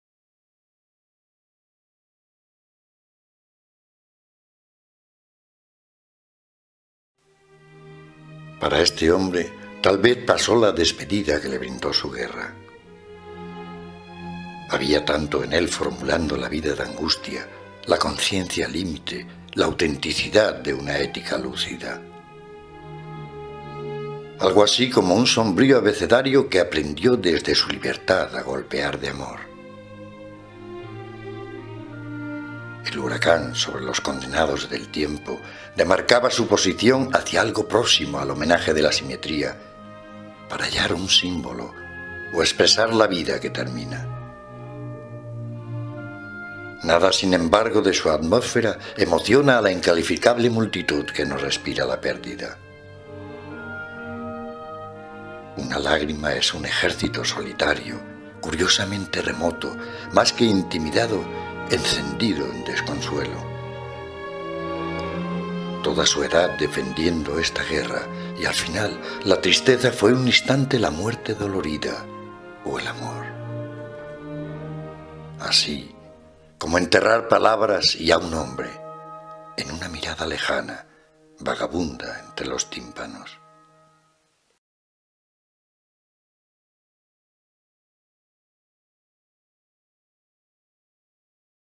ENERO Y NÓMADA.- Una lágrima es un ejército solitario
Vídeo del poema "Una lágrima es un ejército solitario" de José Luis Rico, en la voz del autor